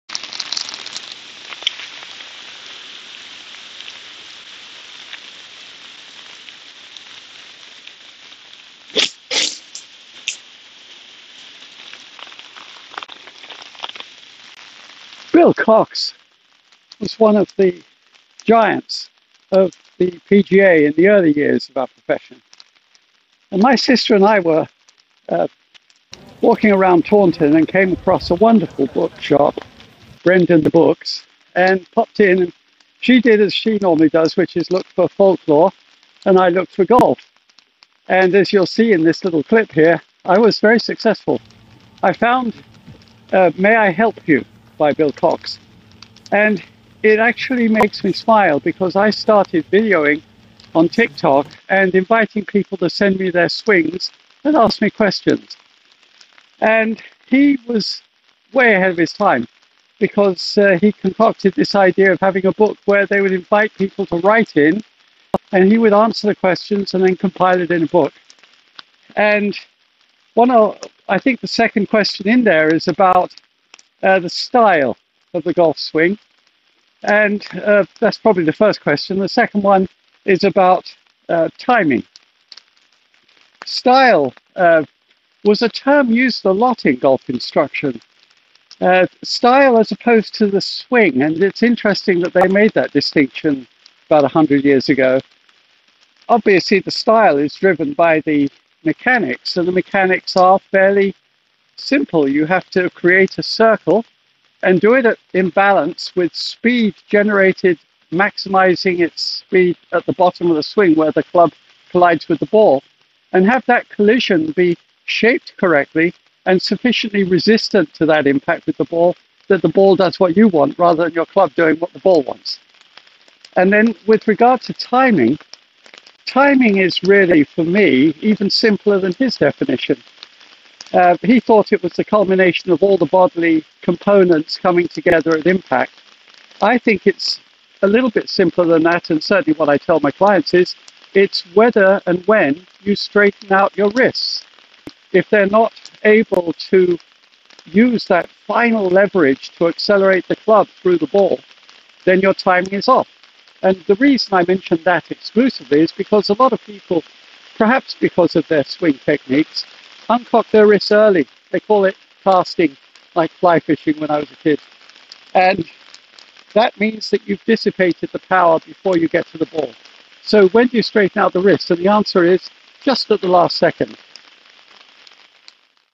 "Can I Help You" by Bill Cox: Style & Timing
⛳ Bill Cox was one of the first giants of the profession and an early member of the British PGA. His book anticipated the influence of social media by asking for and replying to questions about golf. Brit Golf Dad does the same thing on YouTube and Tiktok.
Send a video of your problem shot and hd will reply to those where he believes he can help.